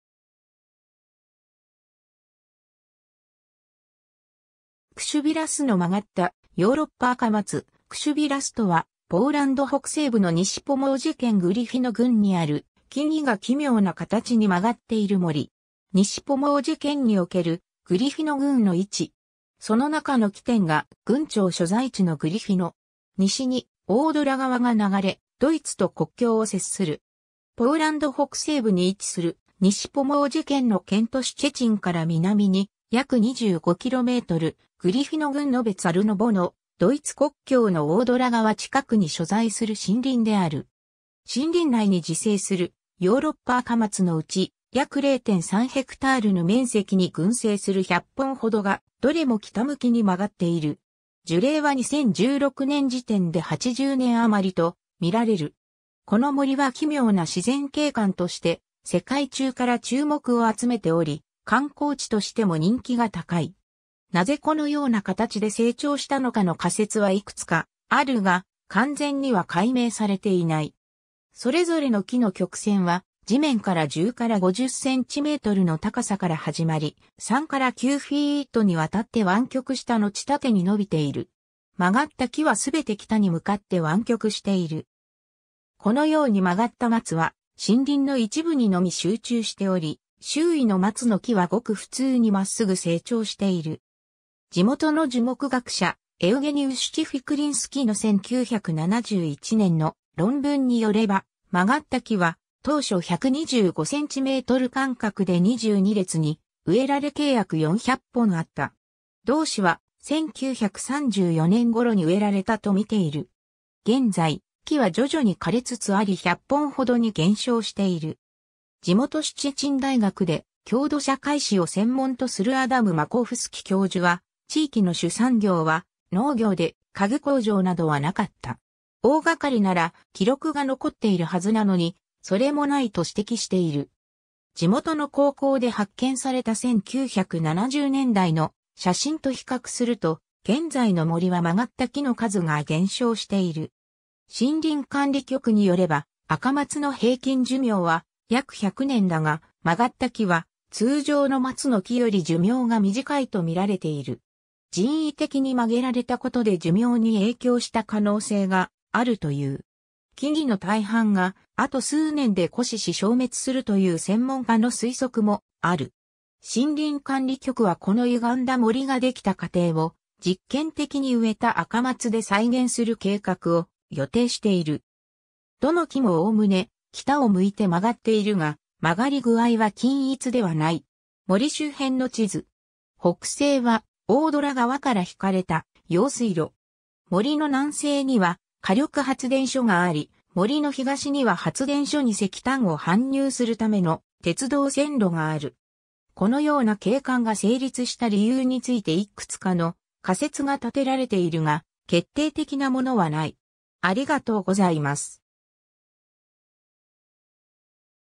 クシュヴィ・ラス
クシュヴィ・ラスの曲がったヨーロッパアカマツ クシュヴィ・ラス（ポーランド語: Krzywy Las、英語: Crooked Forest、直訳で「歪んだ森」）とは、ポーランド北西部の西ポモージェ県グリフィノ郡にある、木々が奇妙な形に曲がっている森。
 西ポモージェ県におけるグリフィノ郡の位置。
その中の黄点が郡庁所在地のグリフィノ。
西にオードラ川が流れ、ドイツと国境を接する。
 ポーランド北西部に位置する西ポモージェ県の県都シュチェチンから南に約25km、グリフィノ郡ノベ・ツァルノボの、ドイツ国境のオードラ川（オーデル川）近くに所在する森林である。
森林内に自生するヨーロッパアカマツのうち、約0.3ヘクタールの面積に群生する100本ほどが、どれも北向きに曲がっている。
樹齢は2016年時点で80年余と見られる。
この森は奇妙な自然景観として世界中から注目を集めており、観光地としても人気が高い。
なぜこのような形で成長したのかの仮説はいくつかあるが、完全には解明されていない。
 それぞれの木の曲線は地面から10～50cm（4～20インチ）の高さから始まり、3～9フィート（最大約3m）にわたって湾曲したのち縦に伸びている。
曲がった木はすべて北に向かって湾曲している。
このように曲がった松は森林の一部にのみ集中しており、周囲の松の木はごく普通に真っすぐ成長している。
 地元の樹木学者エウゲニウシュ・チフィクリンスキの1971年の論文によれば、曲がった木は当初125cm間隔で22列に植えられ計約400本あった。
同氏は「1934年ごろに植えられた」と見ている。
現在、木は徐々に枯れつつあり100本ほどに減少している。
地元シュチェチン大学で郷土社会史を専門とするアダム・マコフスキ教授は「地域の主産業は農業で、家具工場などはなかった。大がかりなら記録が残っているはずなのに、それもない」と指摘している。
 地元の高校で発見された1970年代の写真と比較すると、現在の森は曲がった木の数が減少している。
森林管理局によればアカマツの平均寿命は約100年だが、曲がった木は通常の松の木より寿命が短いと見られている。
人為的に曲げられたことで寿命に影響した可能性があるという。
木々の大半があと数年で枯死し消滅するという専門家の推測もある。
森林管理局はこの歪んだ森ができた過程を実験的に植えたアカマツで再現する計画を予定している。
 どの木もおおむね北を向いて曲がっているが、曲がり具合は均一ではない。
 森周辺の地図。
北西はオードラ川から引かれた用水路。
森の南西には火力発電所 (en) があり、森の東には発電所に石炭を搬入するための鉄道線路がある。
 このような景観が成立した理由についていくつかの仮説が立てられているが、決定的なものはない。